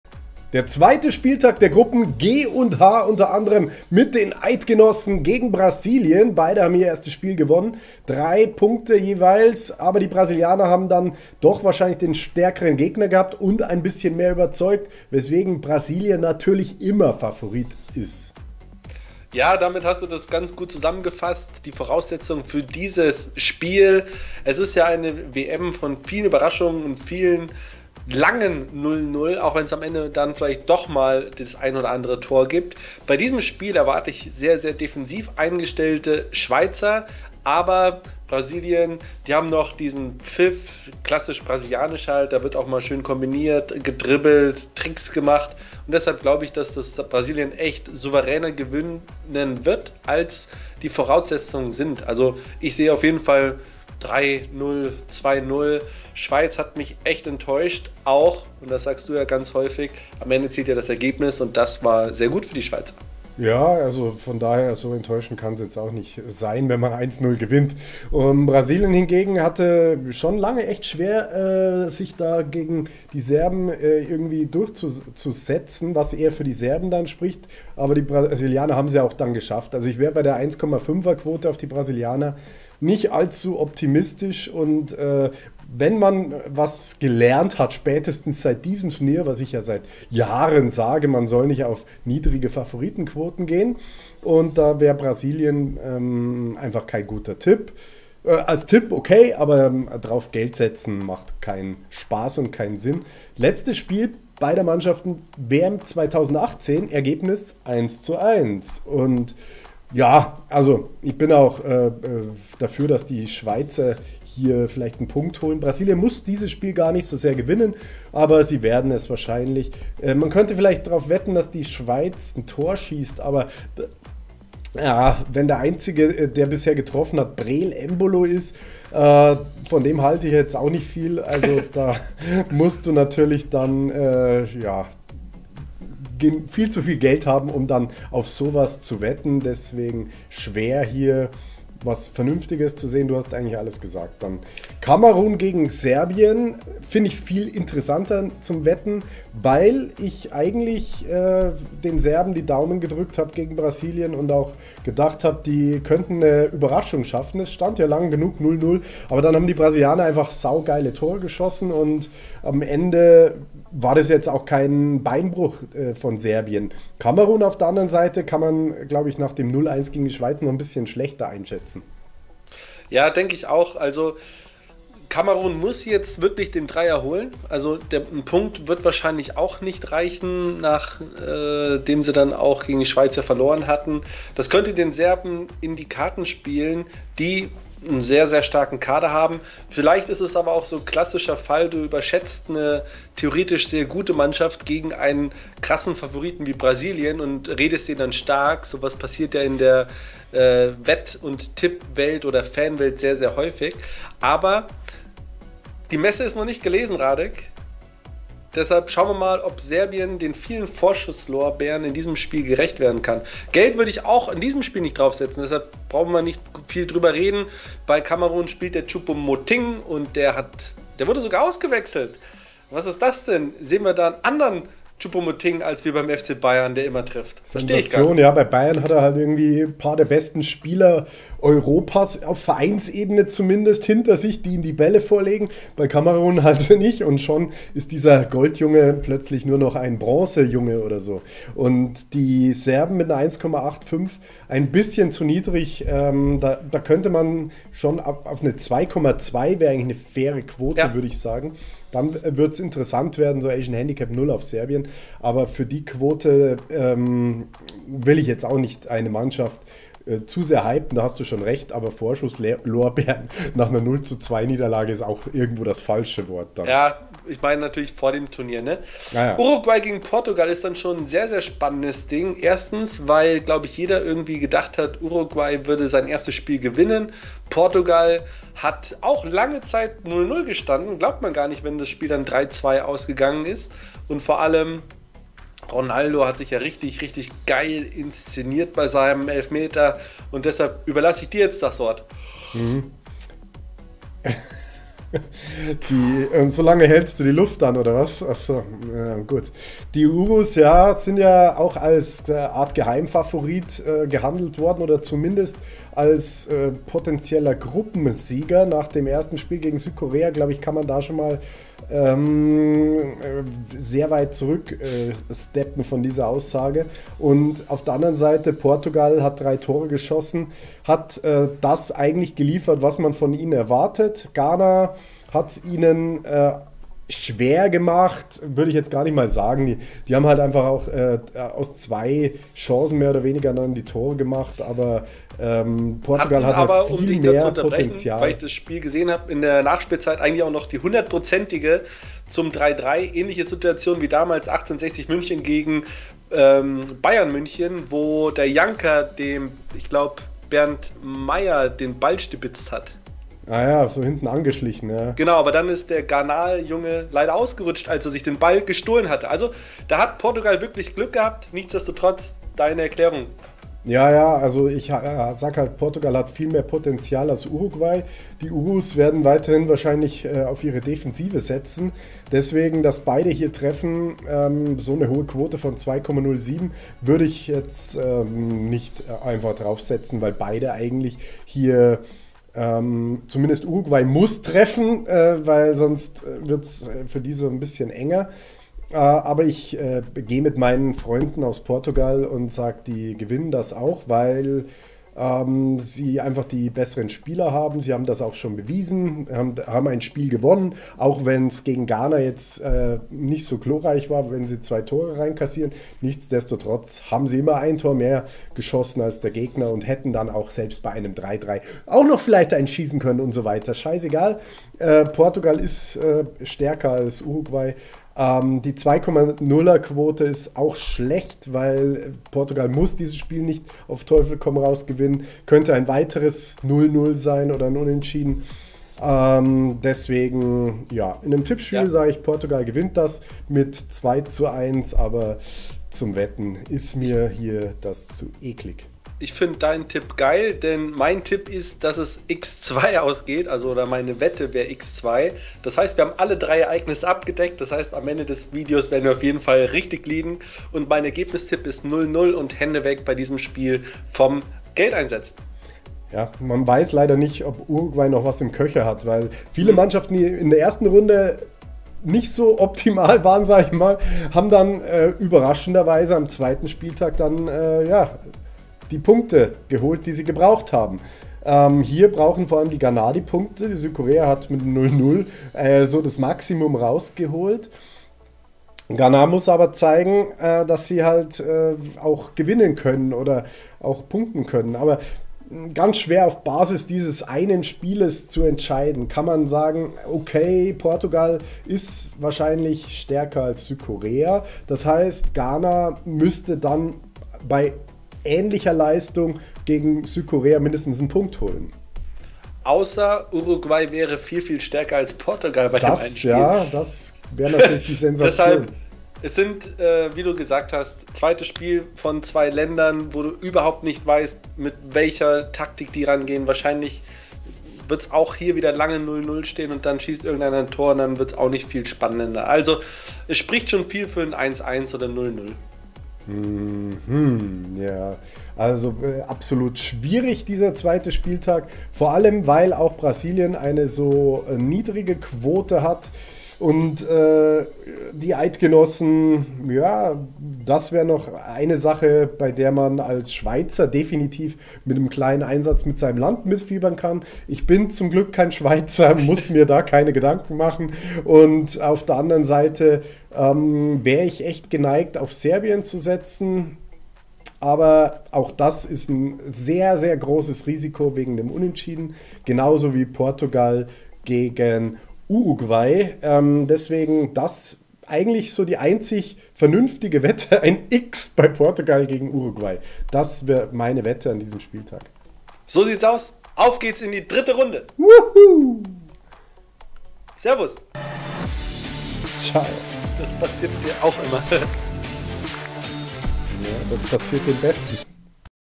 WM-TIPPS heute 🏆 2. Spieltag G+H (Brasilien - Schweiz | Uruguay - Portugal)
Hier ist unsere WM-Prognose mit Wett-Tipps zum 2. Spieltag der Gruppen G und H u.a. mit dem Duell Brasilien - Schweiz. JETZT QUOTEN VERGLEICHEN

00:00 WM-Tipps Schweiz - Brasilien
02:24 Kamerun - Serbien (Prognose)
04:29 Uruguay - Portugal (RadeX Bet)
07:52 Südkorea - Ghana (Ach komm!)